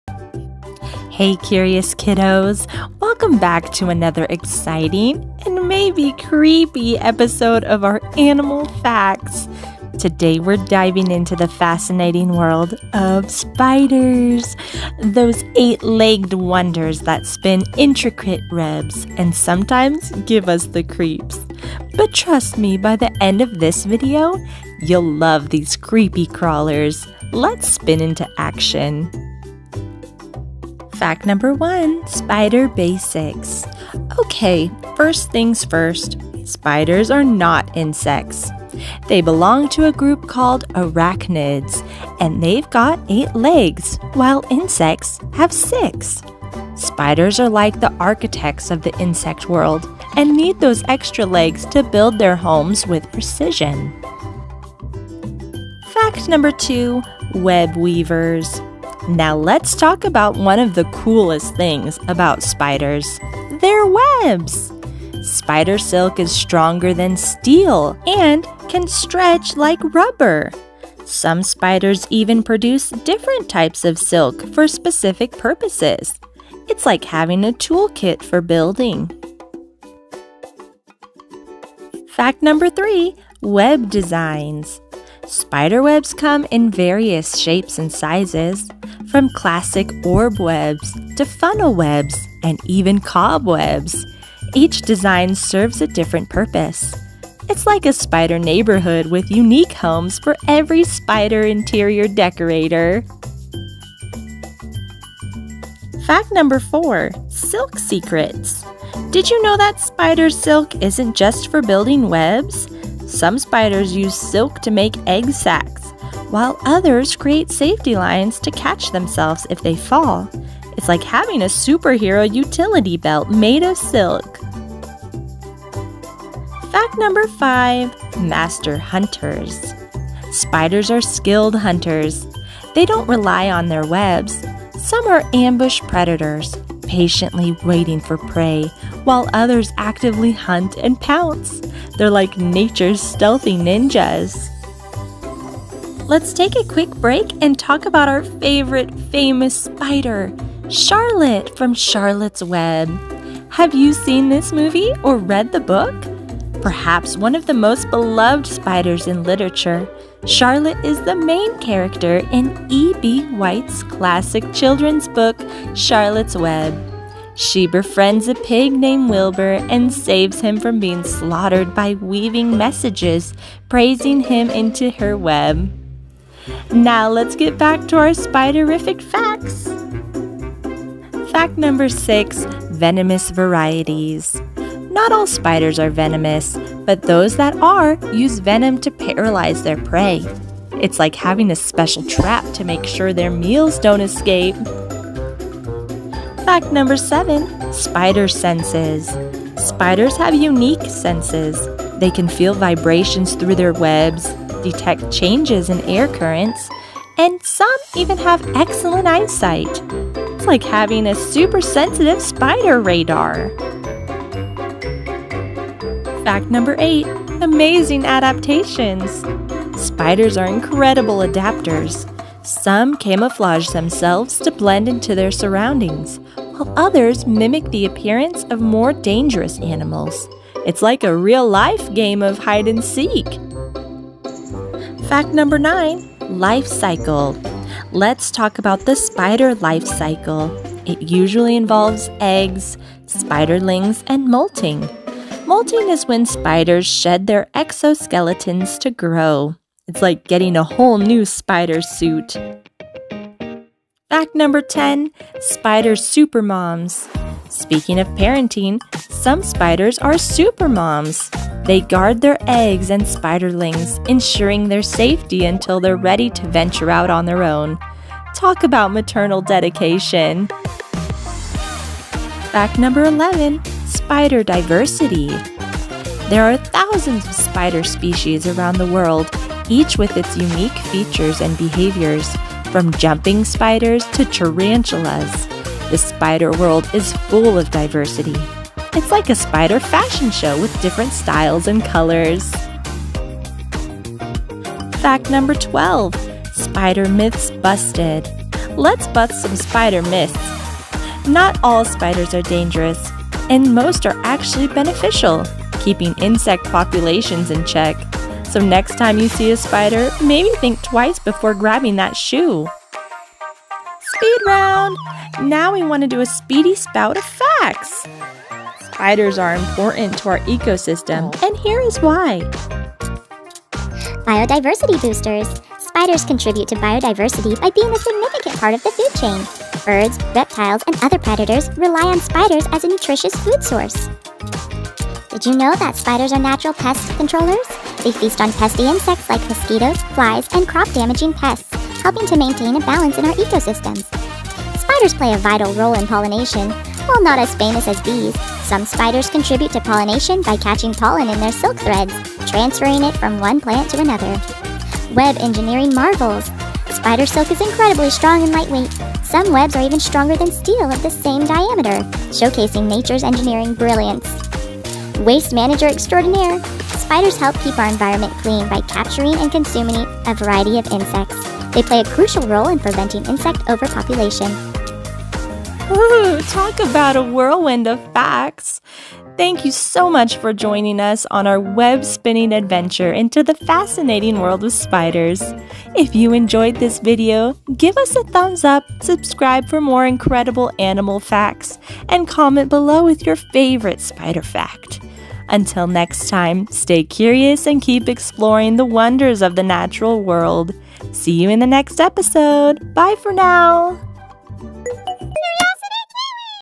CREEPY Spider Facts! 😱🕷🕸 Discover the Spider with us! Animal Facts for Kids | Educational Facts
Join us for a creepy, crawly episode of animal facts! Today we unravel the web of facts about spiders! Make sure you watch the whole video for our fun speed round!

Music from Uppbeat (free for Creators!)